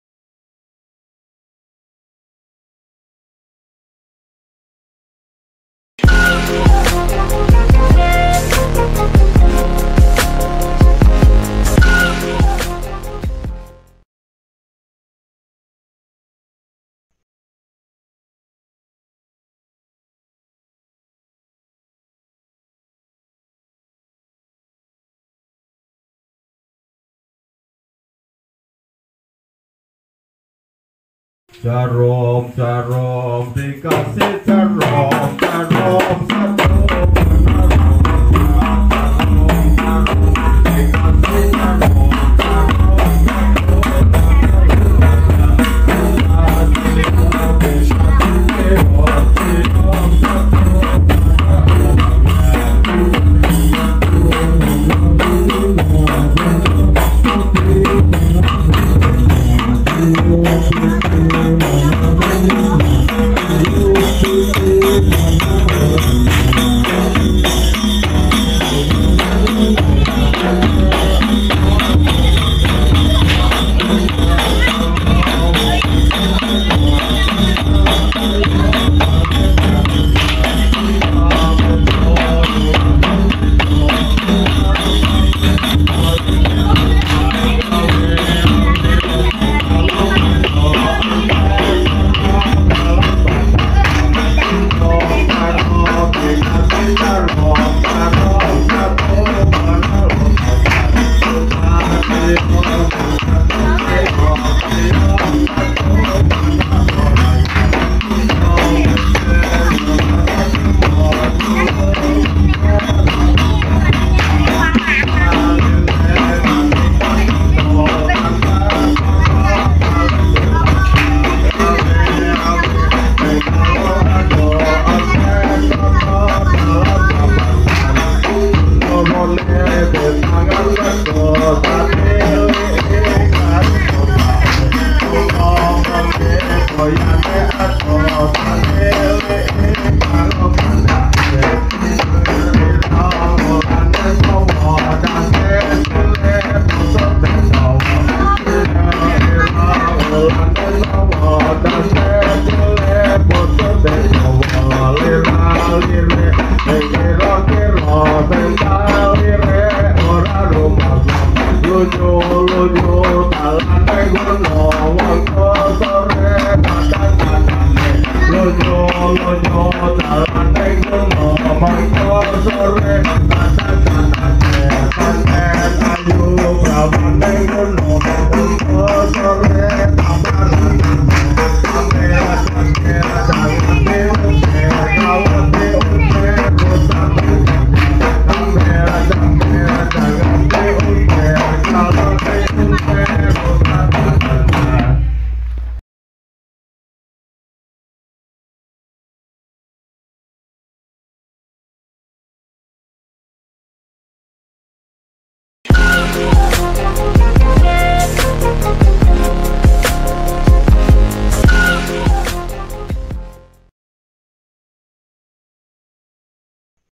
TARI JARUM JARUM, MERAWAT SENI TRADISI : 76. PARYONO, S.Pd (PURWOREJO)
Tari Jarum Jarum - Grup Dolalak Sri Utami. Ditayangkan tanggal 30 November 2020 pukul 14.00 WIB sampai dengan tanggal 10 Desember 2020 pukul 14.00 WIB. Akan dipilih 10 finalis berdasarkan jumlah penonton terbanyak. Kemudian dari para finalis tersebut, juaranya akan dipilih oleh Dewan Juri.